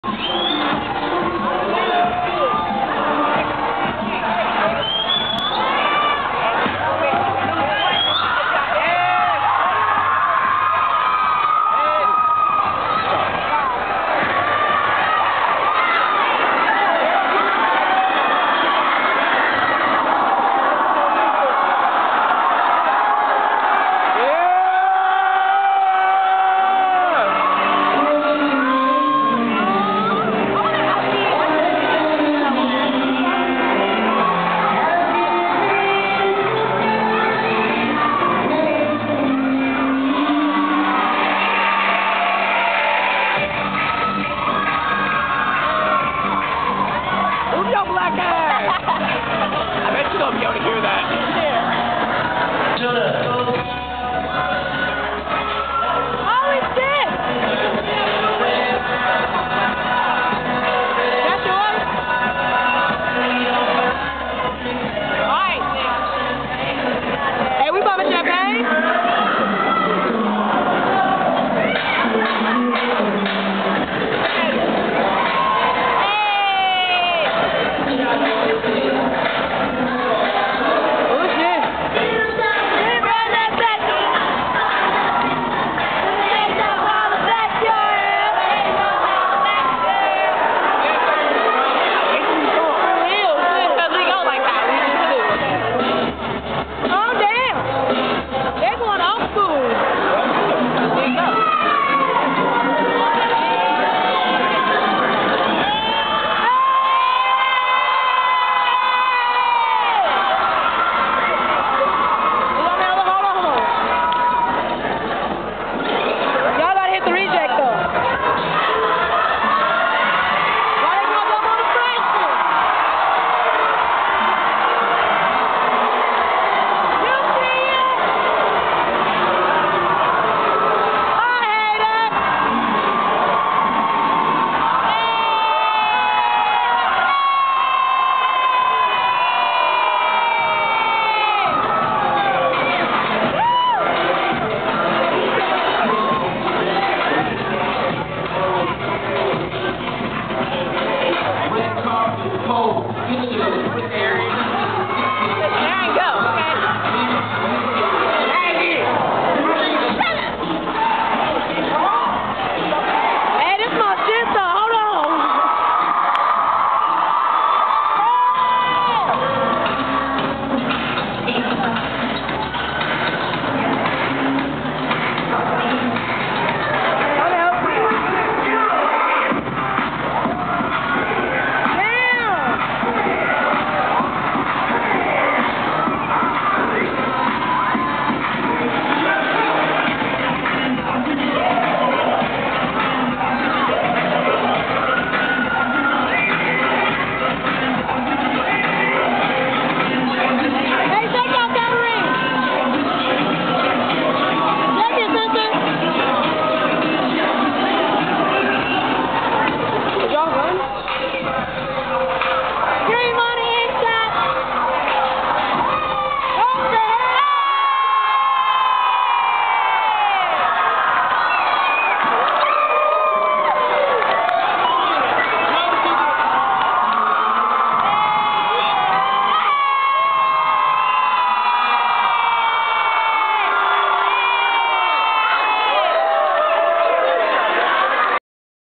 hayward high sophomore dtr dance
hayward high sophomore dance.we beat the class of 2011 once again!! our theme was pac-man.